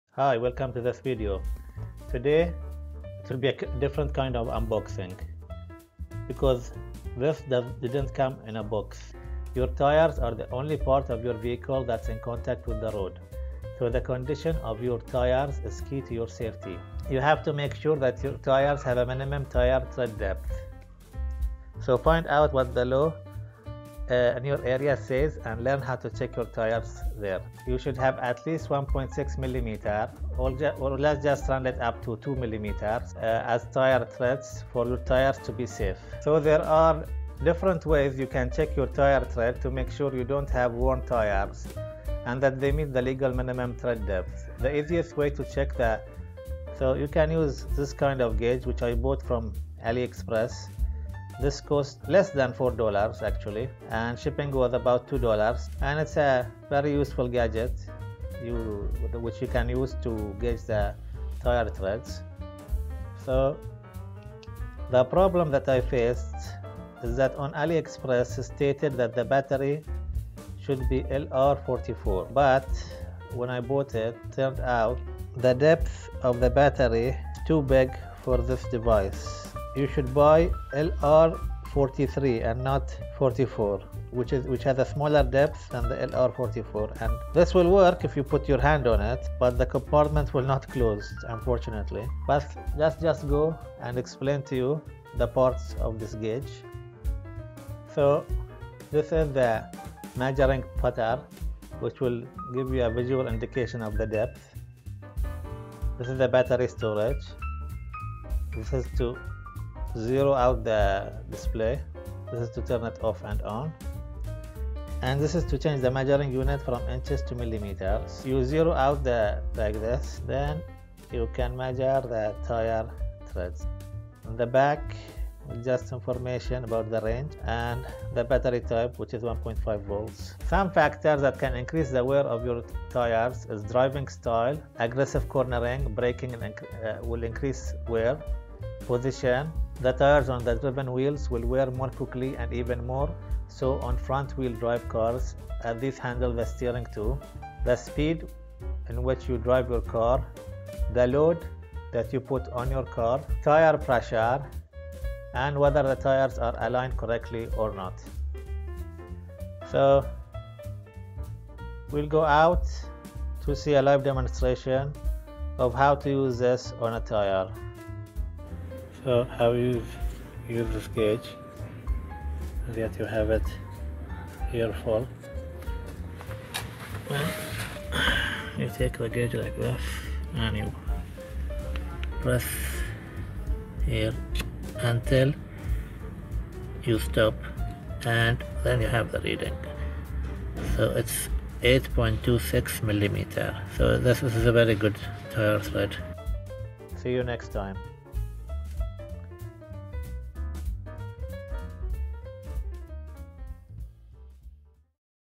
Importance and Use of a Digital Tyre Tread Guage
You need to measure your tire's tread depth regularly to ensure your tires can still grip the road. There are a couple of ways you can accomplish this: First, by using a tread depth gauge, and second, by looking at a tire's built-in tread indicators.

In this video I will review a Digital Tyre (Tire) Tread Guage and explain why it is important to measure it especially of your tyre is older than five year.